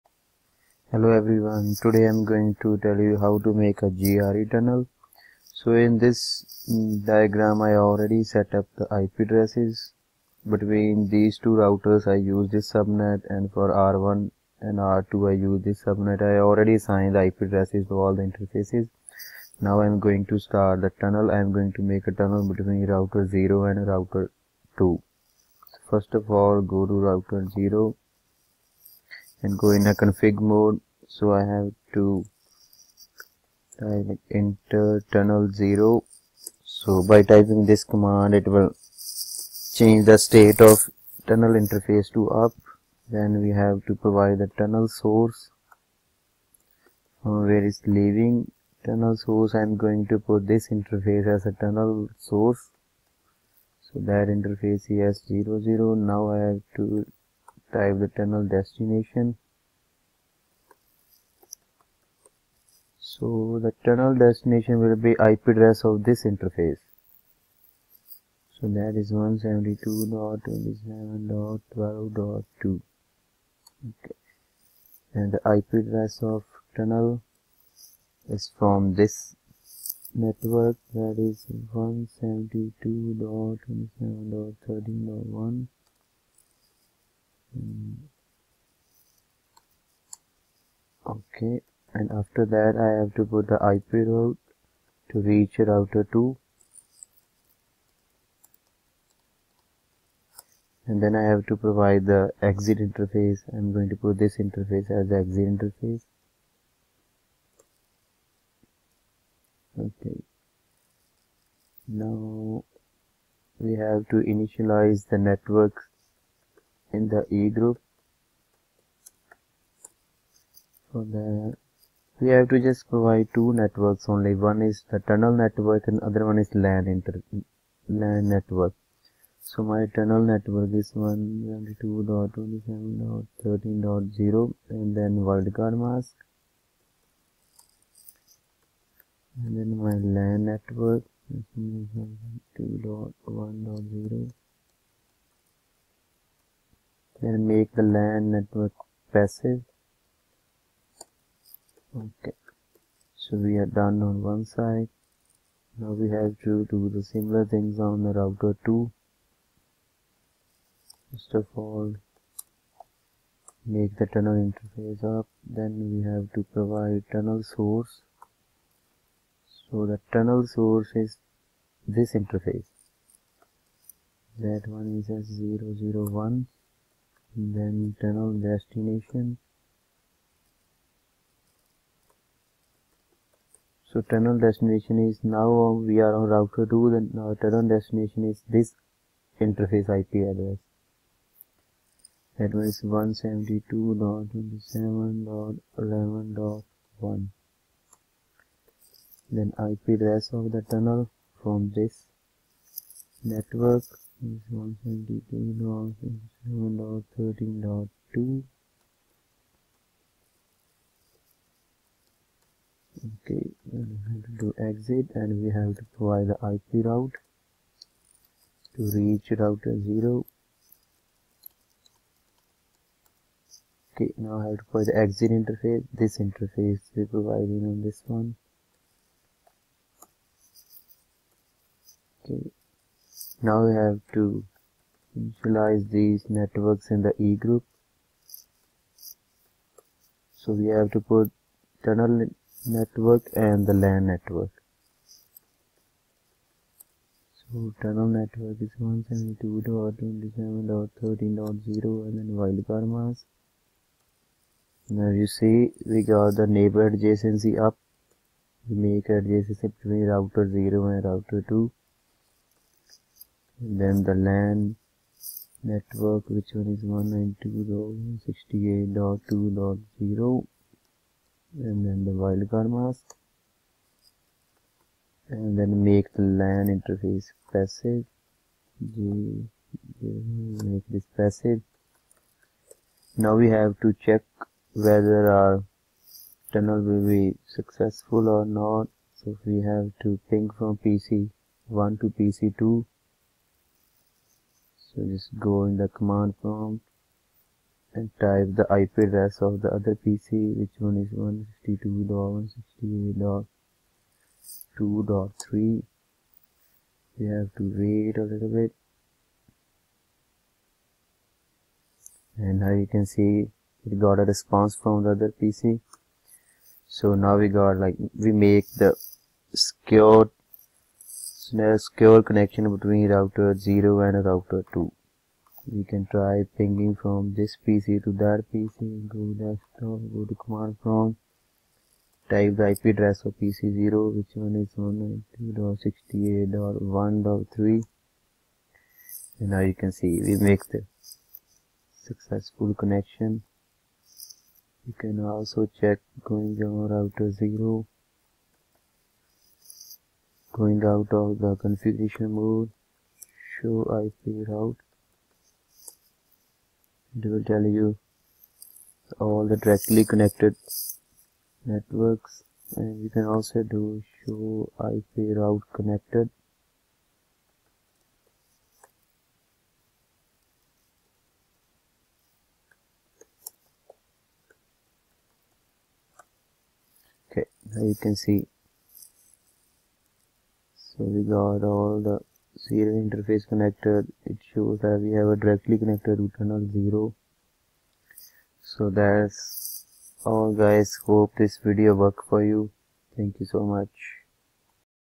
how to make GRE tunnel
This video will help in configuring secure connection between two routers with the help of GRE tunnel